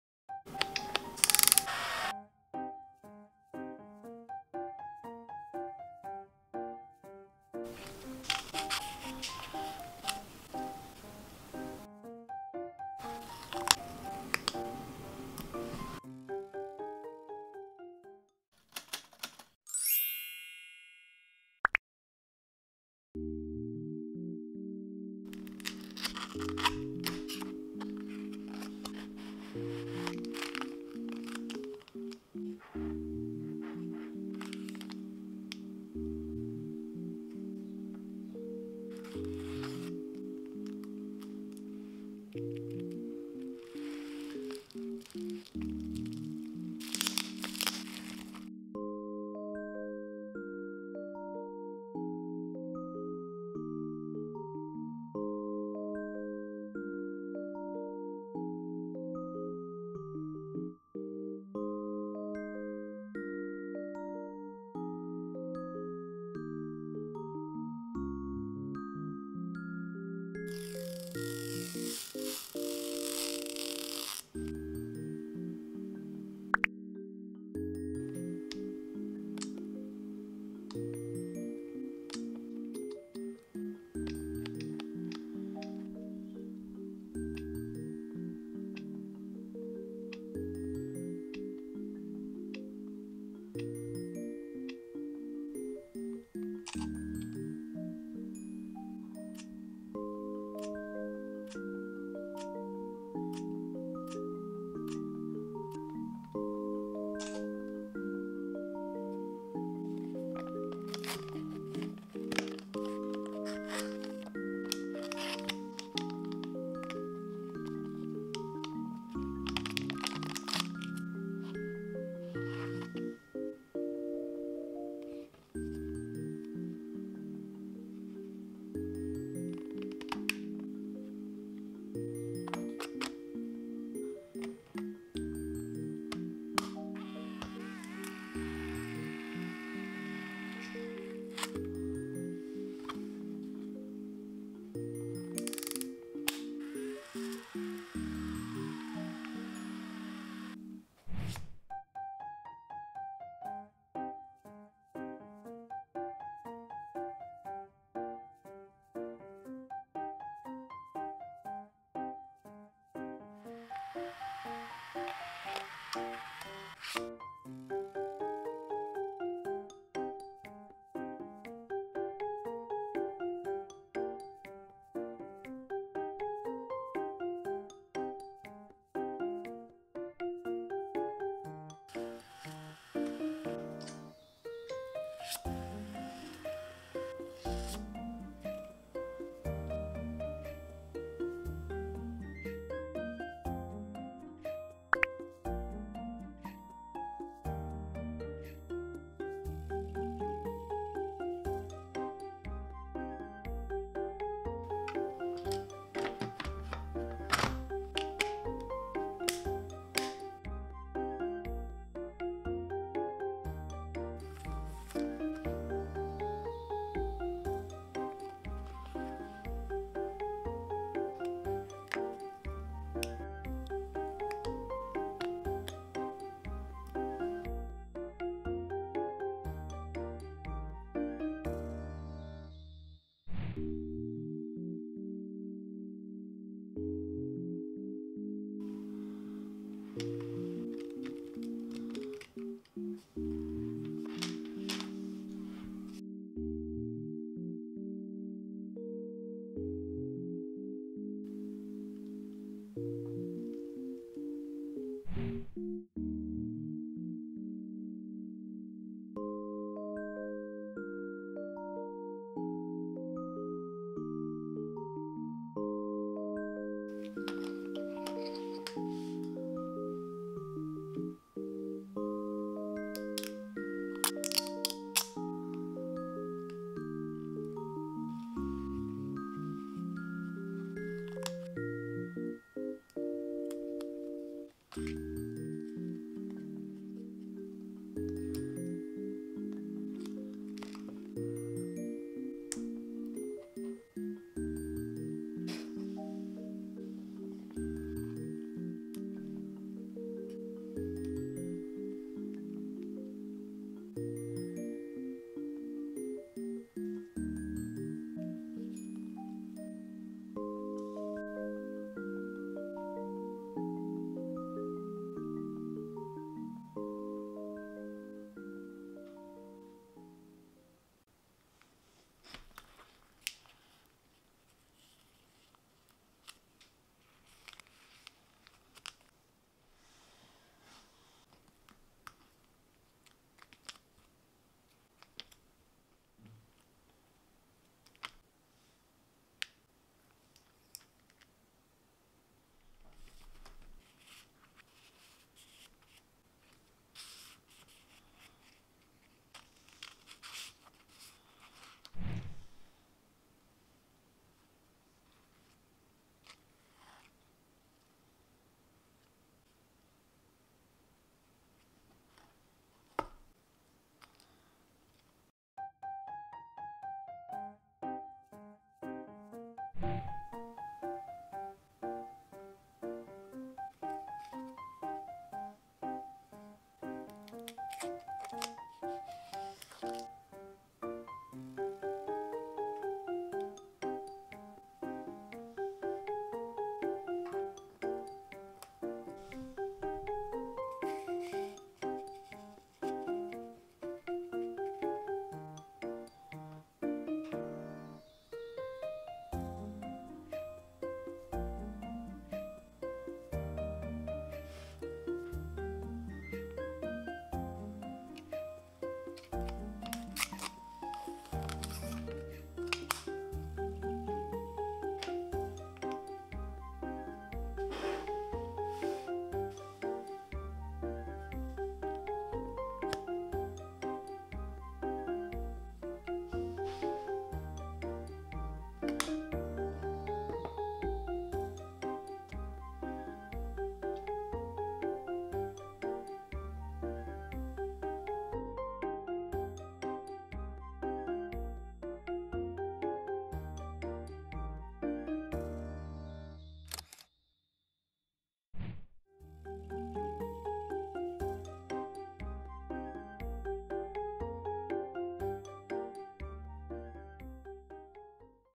✧Fujifilm Instax mini Evo unboxing: ✿adding a touch of cuteness with accessories from AliExpress✿
૮₍ ´• ˕ •` ₎ა
Hi there! ❤︎
I recently bought this Fujifilm Instax mini Evo  (ﾉ≧∀≦)ﾉ. I bought this after contemplating it a lot. I also bought lovely accessories from AliExpress to go with it. Enjoy the video! ♡( ⁎ᵕᴗᵕ⁎ )
Nayo✦

✧Here is the link to the items✧

✦ camera case
✦ stickers ( peaches and bees )
✦ beaded bow knot strap
✦ checkered photo card album
✦ flower photocard album
✦ acrylic diy pendant
✦ cute bear acrylic frame
✦ transparent magnet frame